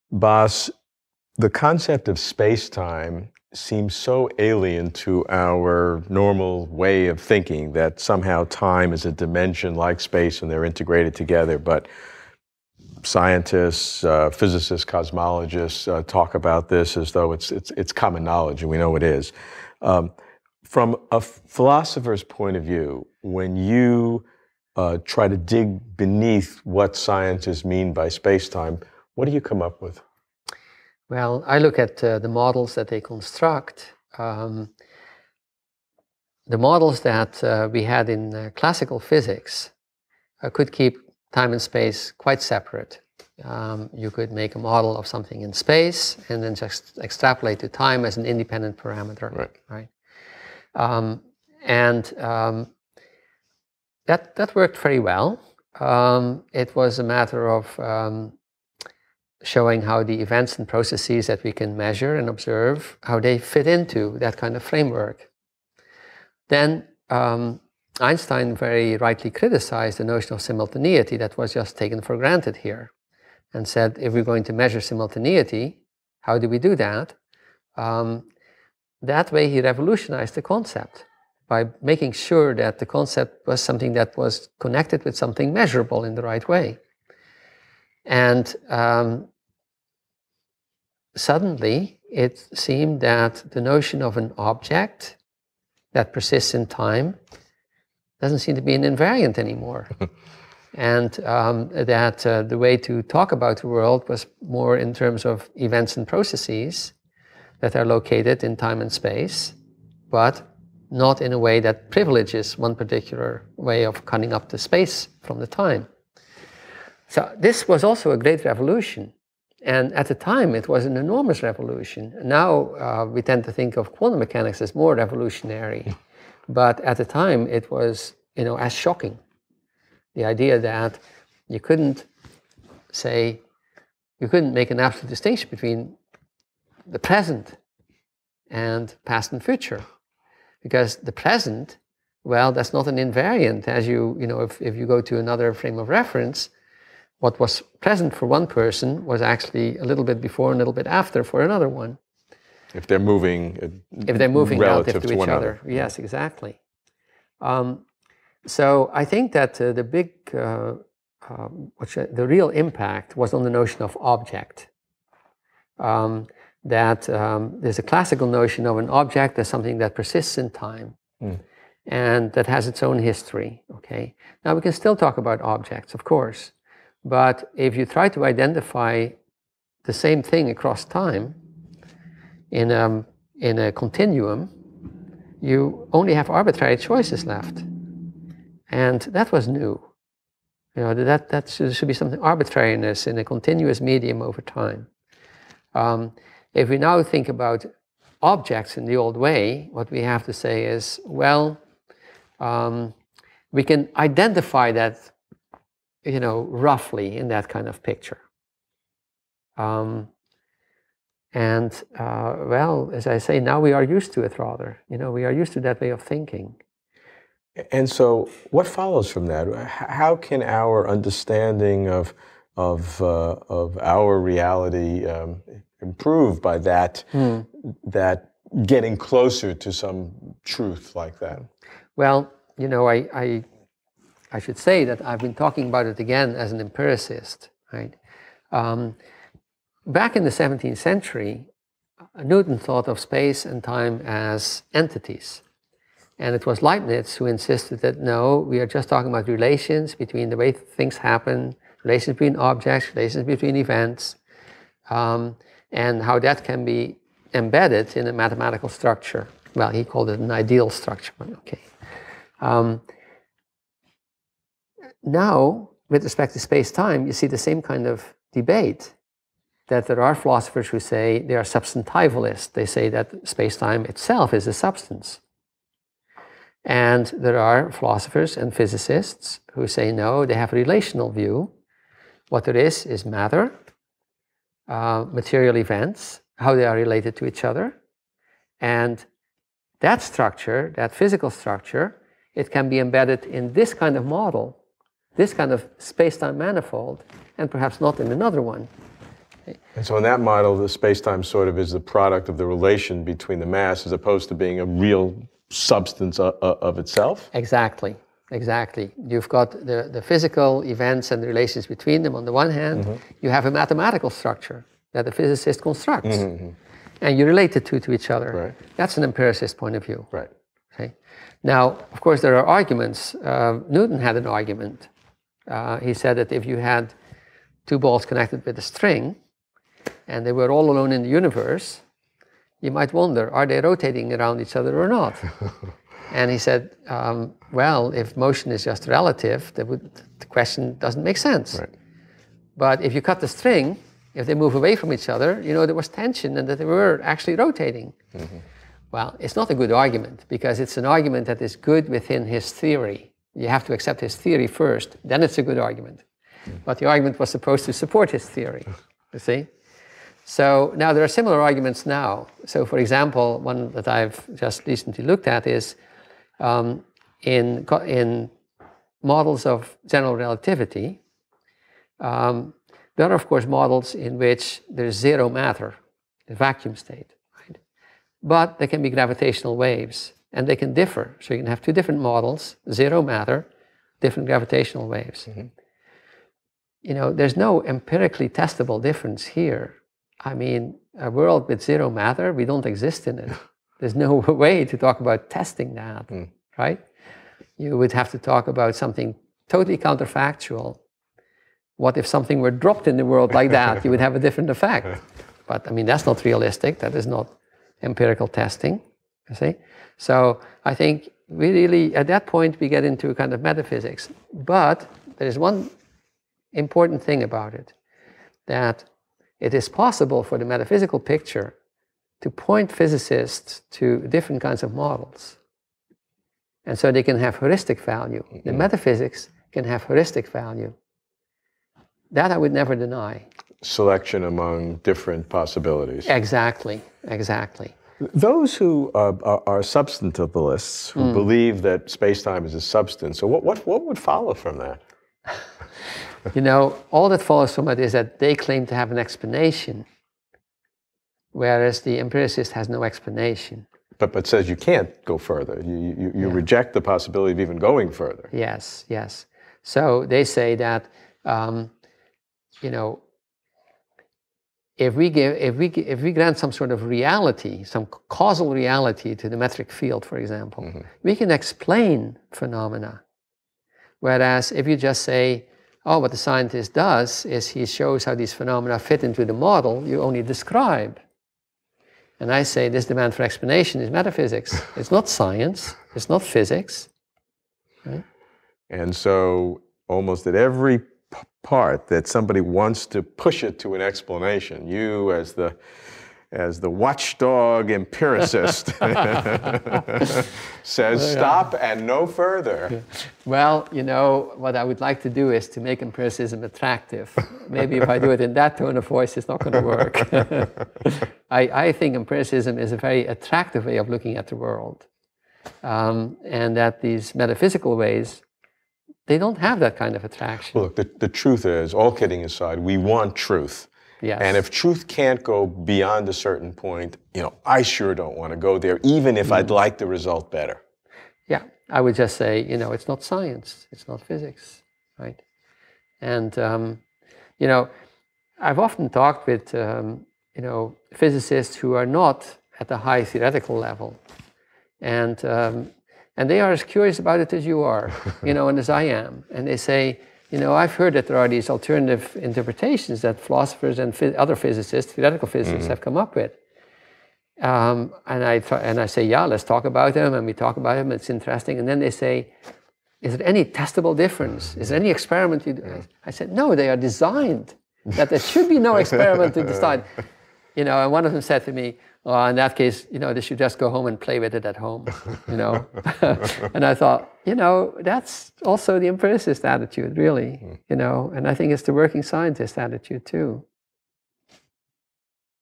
Bas van Fraassen - How Can Space and Time be the Same Thing?
What does it mean for space and time to be the same thing? Not related to each other, but literally two descriptions of precisely the same entity called ‘spacetime’? One cannot understand existence without understanding spacetime.

Bas C. van Fraassen is a Distinguished Professor of Philosophy at San Francisco State University and the McCosh Professor of Philosophy Emeritus at Princeton University. He teaches courses in the philosophy of science, philosophical logic and the role of models in scientific practice.

Get free access to Closer to Truth's library of 5,000 videos